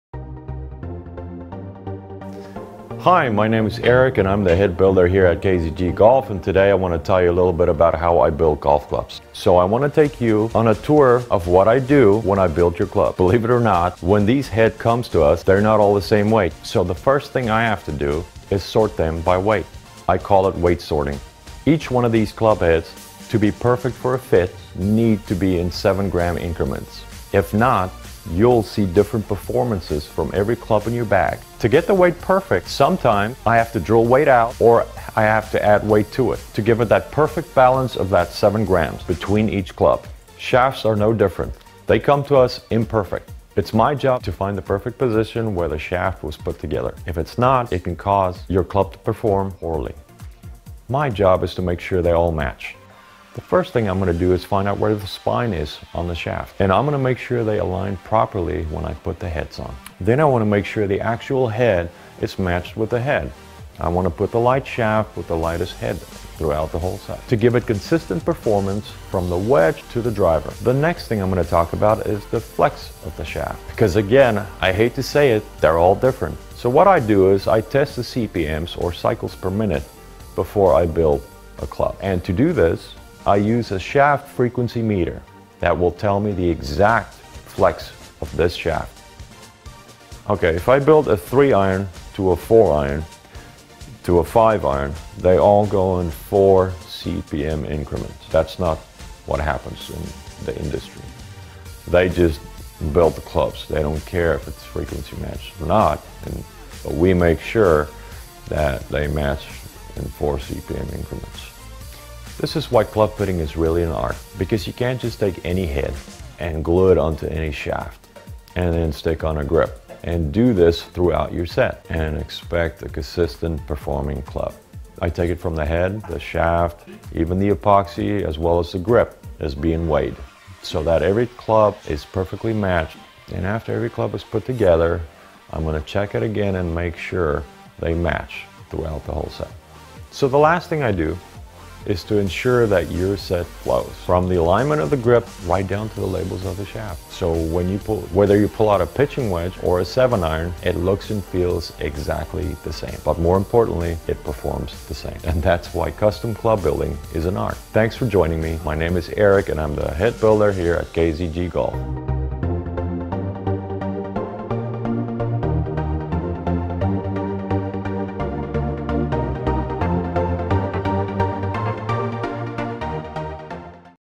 How do you build Golf Clubs?
I want to take you on a tour of what I do when I build your club. Believe it or not, when these heads come to us they are not all the same weight. The first thing I have to do is sort them by weight. I call it weight sorting. Each one of these club heads to be perfect for a fit need to be in 7g increments. If not, you’ll see different performances from every club in your bag. To get the weight perfect, sometimes I have to drill weight out or I have to add weight to it to get that perfect balance of that 7g between each club.